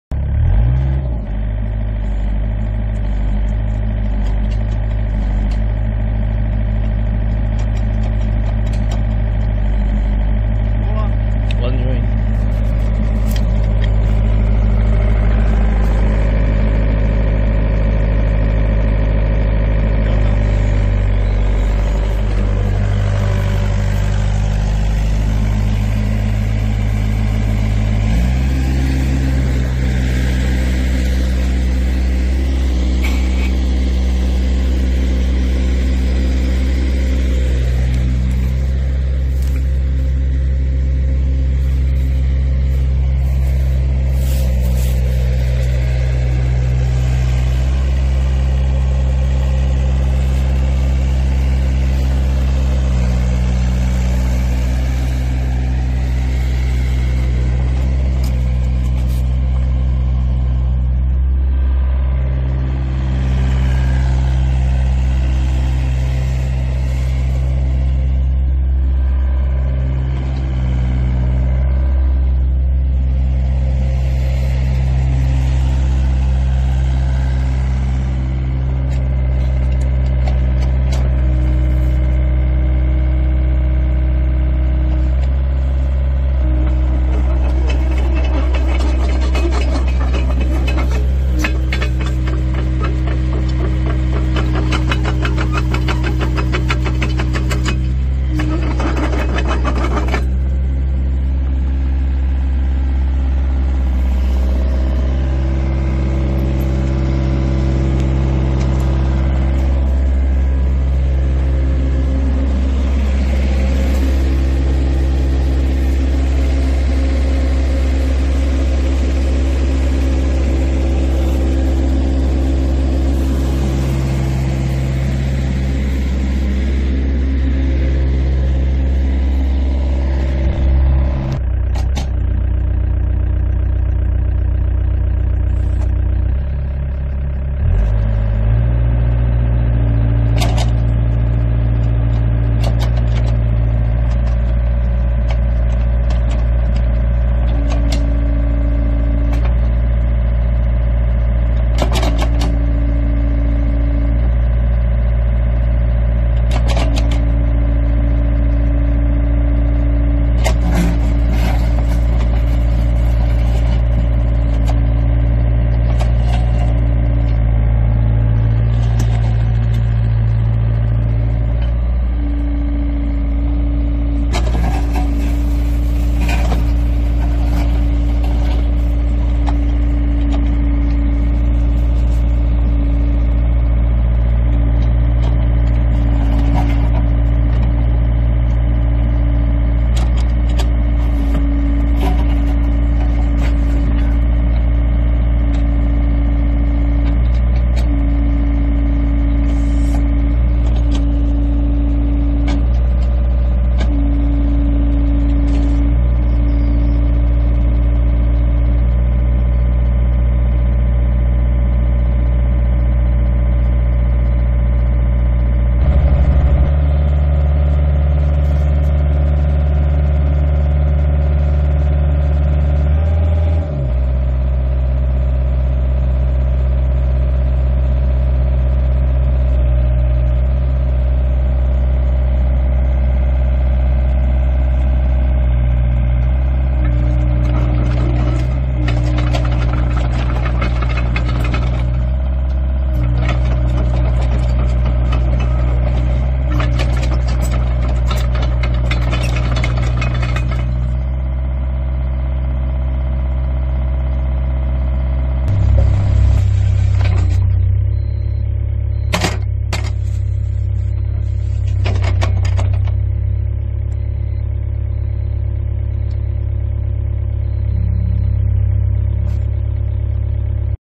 볼보 농장용 굴삭기 EC55C 2008년 코끼리 집게 장착 자가용으로 적극 추천!
★ 제조사 : 볼보
★ 모델명 : EC55C
★ 년  식 : 2008년
★ 가동시간 : 15497시간
★ 구  성 : 본 바가지 + 코끼리 집게
★ 위  치 : 경북 고령

★ 미니~10급/W 굴삭기 정보,가격,시운전 ★

내차시세?매입?

중고굴삭기 매입,판매 전문 이용재 과장